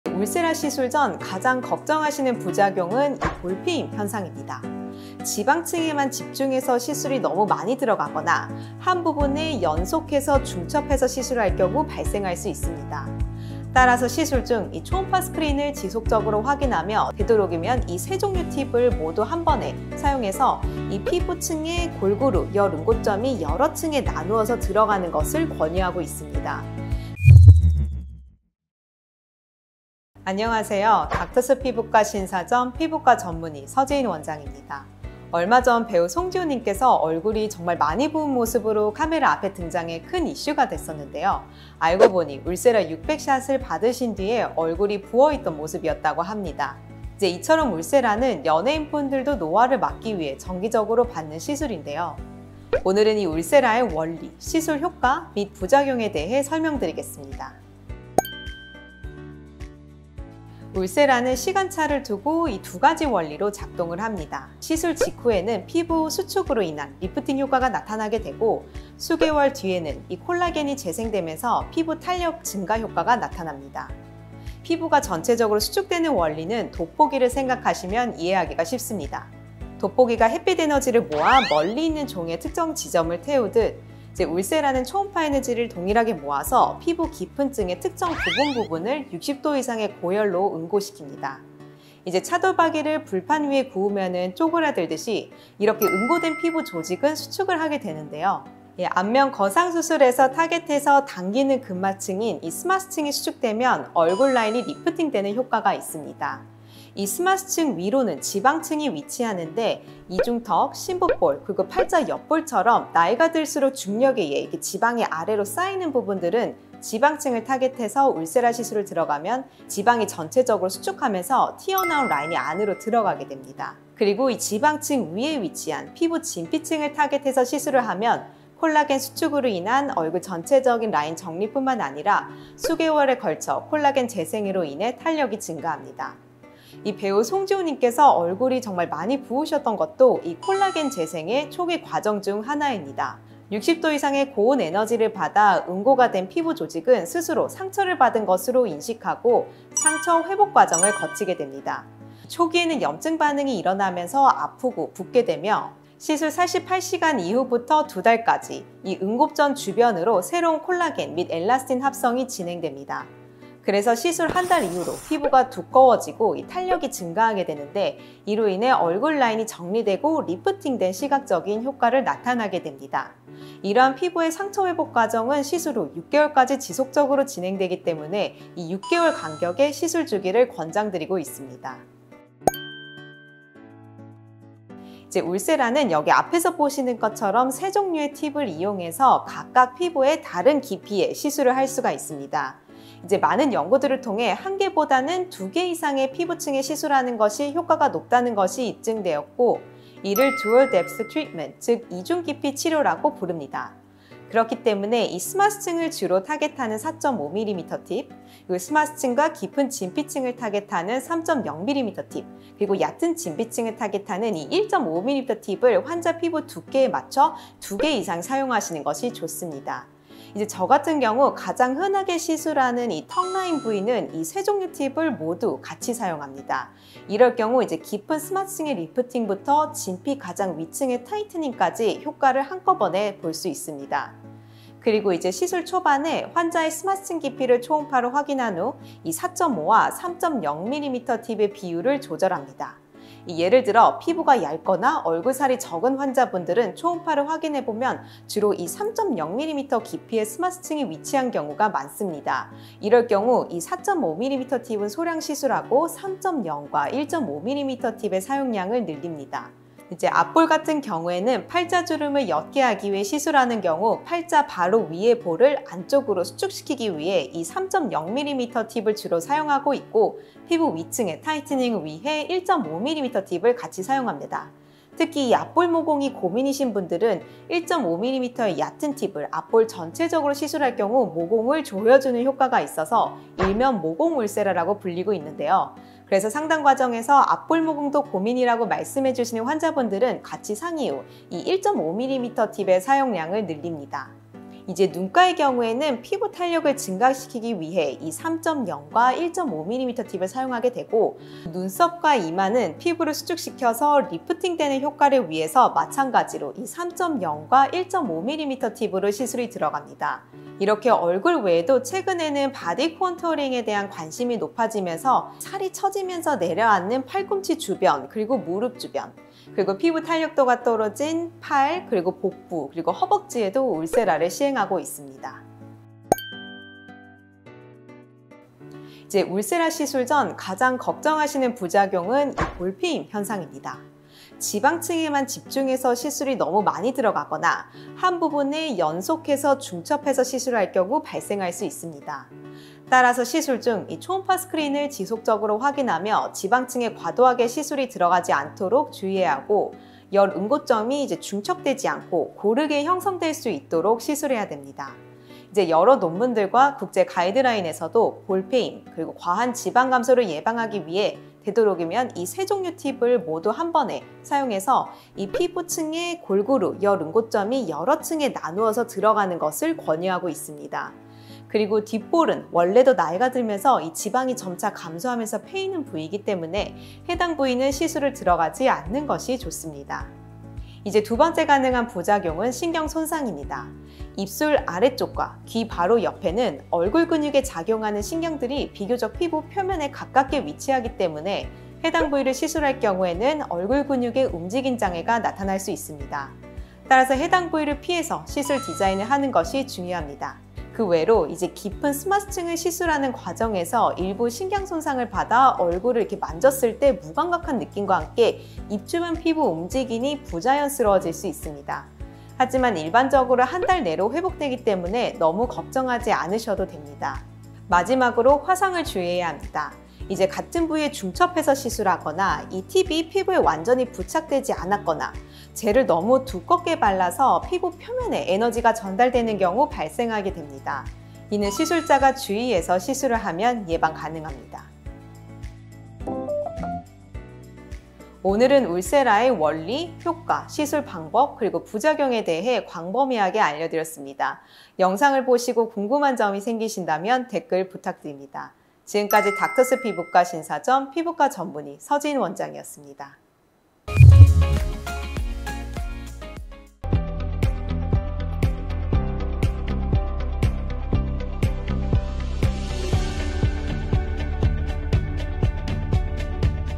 연예인 필수 관리 울쎄라! 꿀팁 알고 받아야죠😉
최근 배우 송지효님도 울쎄라를 받고 퉁퉁 부은 붓기부터 갸름해진 얼굴까지! 가장 많은 이슈였죠🫢 울쎄라 장인 닥터스피부과에서 찐 꿀팁 다 알려드립니다❗
제대로 알고 받아야 2배 효과를 기대해볼 수 있어요!!

볼패임 없이 3가지 종류의 팁을 골고루 사용하여 탄력만 되돌리는 V라인 비법 대공개❤️

📢 피부과전문의 서지인 원장님의 울쎄라 총정리 영상에서 확인하세요 

🎬영상 내용🎬
 00:00 하이라이트
 00:30 인트로
 00:58 울쎄라의 원리 및 효과
 03:02 시술 방법 및 부위?
 05:26 울쎄라 부작용은?
 07:17 마무리

모든 피부과 시술은 개인의 피부 상태와 특성에 따라 크고 작은 부작용이 있을 수 있습니다. 시술 전 풍부한 경험을 가진 의료진과 충분히 상담을 거친 후에 신중하게 시술을 결정하시기 바랍니다.